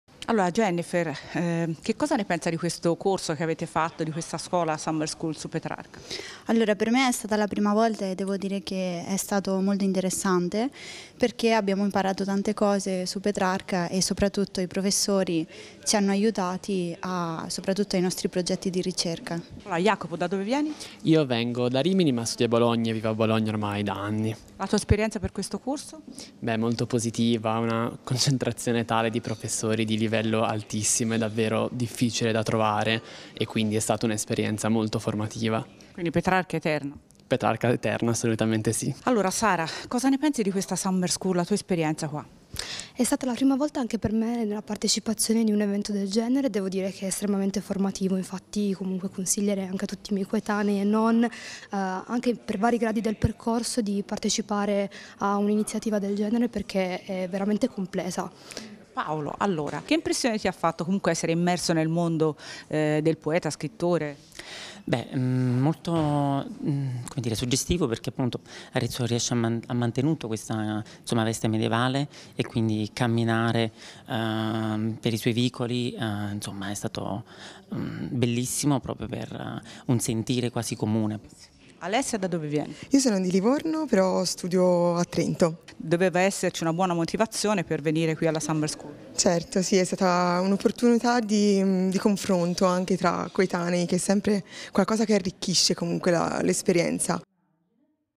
Summer school Petrarca, esperienze
Tornano a casa con un bagaglio di sapere ed emozioni importanti i partecipanti alla Summer School dell'Accademia Petrarca che ha visto intervenire insegnanti da tutto il mondo. Sentiamo i giovani studiosi e le loro esperienze in merito.